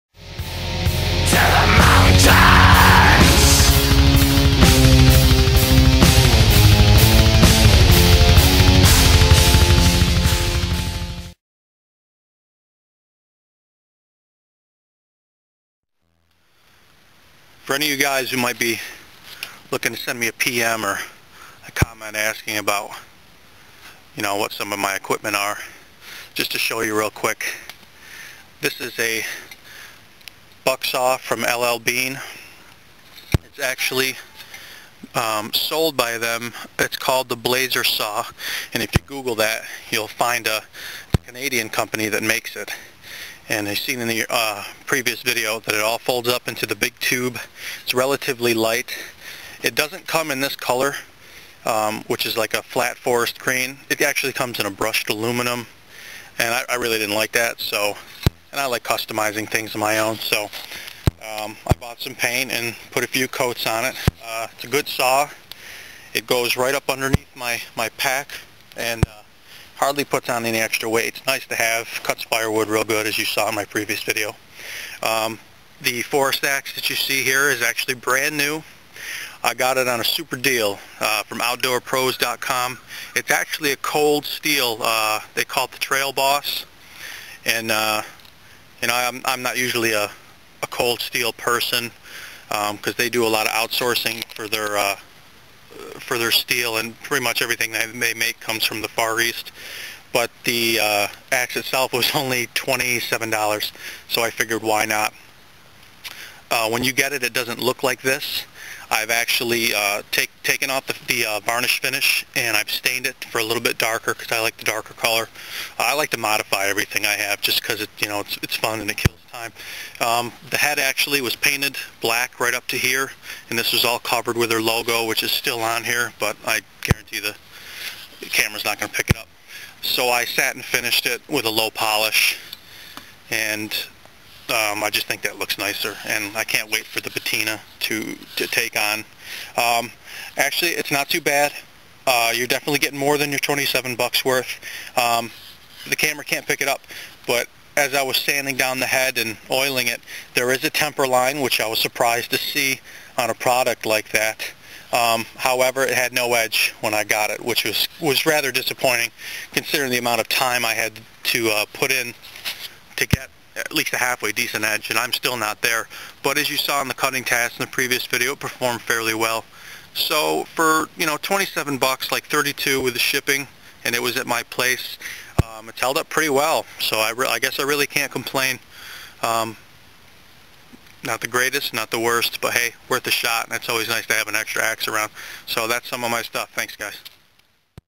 Buck/pack saw and Axe-New kit
Just a real quick review on some new kit.